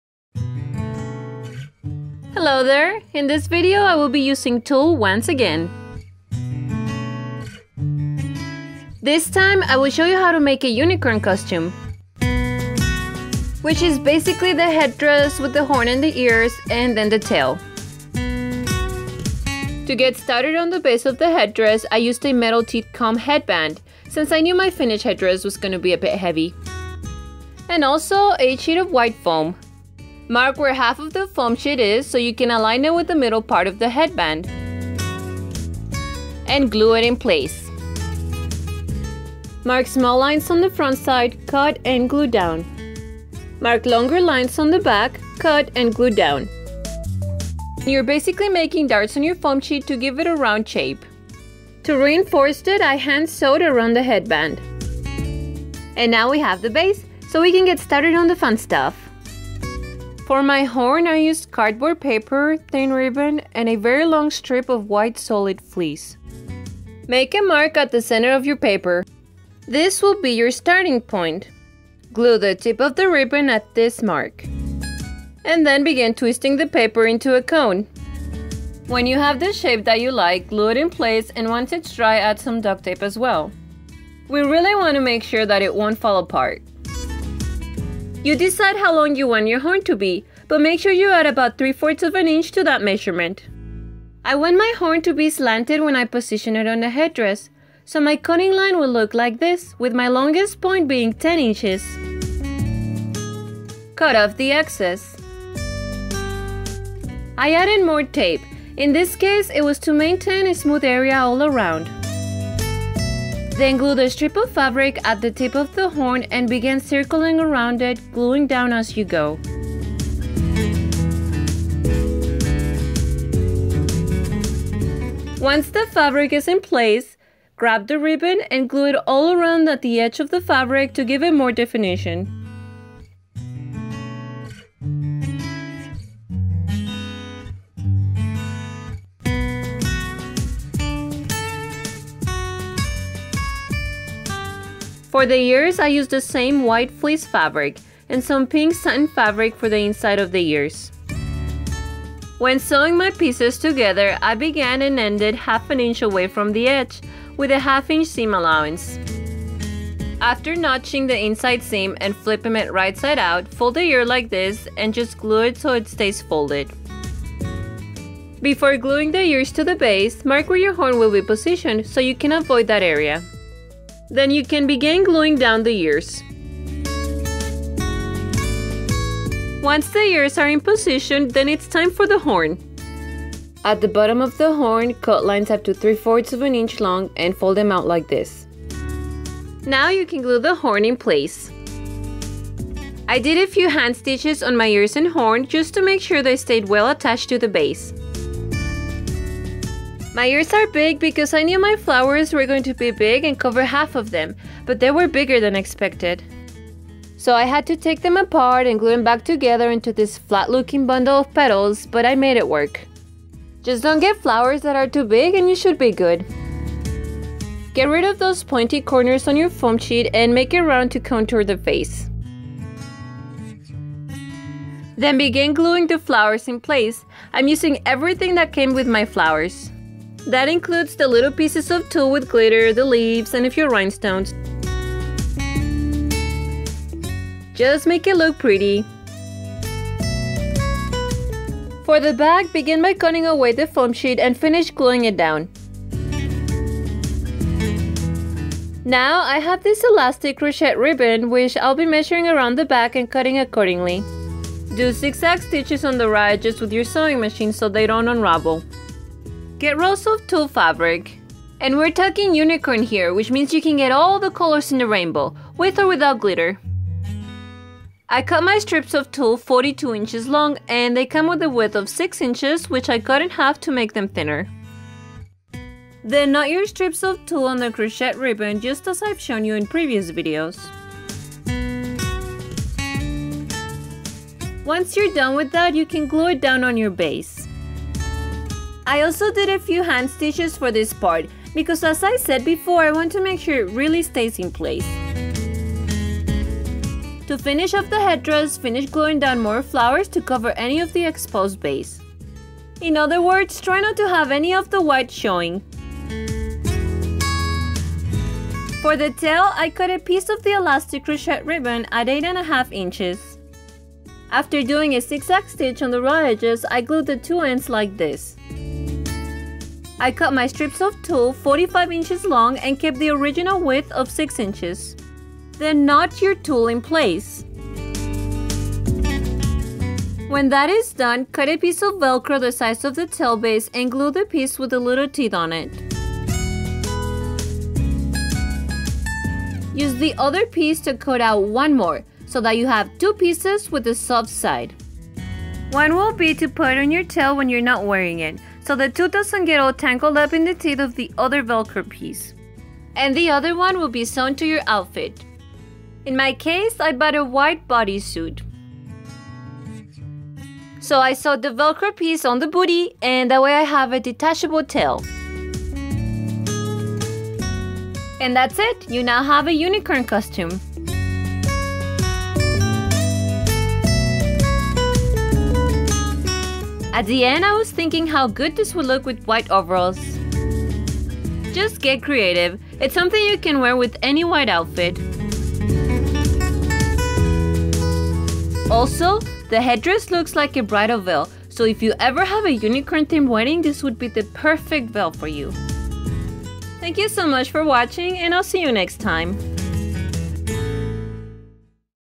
Unicorn Costume DIY (headdress and tail)
How to make a unicorn headdress and a tail.
I used tulle fabric, and it's a project you could do without a sewing machine.
Can be worn with any white outfit and it's simple to make.